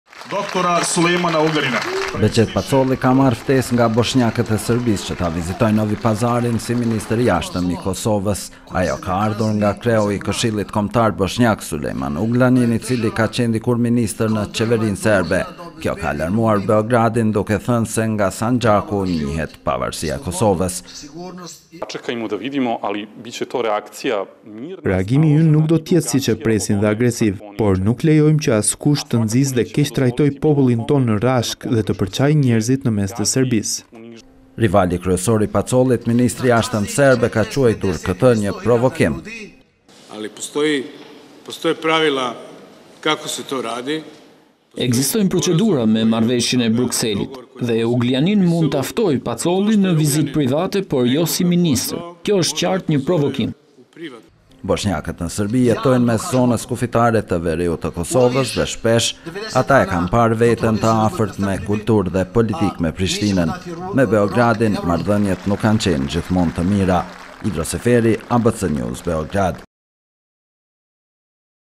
Boshnjaket e Serbise ftojne Pacollin per vizite, reagon Beogradi | ABC News Albania
Lideri i boshnjakëve të Serbisë, që jetojnë në kufirin verior të Kosovës, kanë ftuar për një vizitë ministrin e Jashtëm kosovar, Behgjet Pacolli. I menjëhershëm ka qenë reagimi i Beogradit, që një lëvizje të tillë e ka quajtur provokim dhe një përpjekje për të përçarë Serbinë.

Behgjet Pacolli ka marrë ftesë nga boshnjakët e Serbisë që të vizitojë Novi Pazarin si ministër i Jashtëm i Kosovës. Ajo ka ardhur nga kreu i Këshillit Kombëtar boshnjak, Sulejman Ugljanin, i cili ka qenë dikur ministër në qeverinë serbe. Kjo ka alarmuar Beogradin, që ka thënë se nga Sanxhaku njihet pavarësia e Kosovës.

Marko Gjuriç, Zyra për Kosovën në Serbi: “Reagimi ynë nuk do të jetë agresiv, siç presin disa. Por nuk lejojmë që askush të nxisë dhe keqtrajtojë popullin tonë në Rashkë dhe të përçajë njerëzit në mes të Serbisë”.

Rivali kryesor i Pacollit, ministri i Jashtëm serb, Ivica Daçiç e ka quajtur këtë një provokim.

Ivica Daçiç, ministër i Jashtëm i Serbisë: “Ekzistojnë procedura në marrëveshjen e Brukselit dhe Ugljanin mund ta ftojë Pacollin për një vizitë private, por jo si ministër. Kjo do të ishte një provokim i qartë”.

Boshnjakët në Serbi jetojnë mes zonës kufitare të veriut të Kosovës dhe shpesh ata e kanë parë vetën të afërt me kulturë dhe politikë me Prishtinën./abcnews.al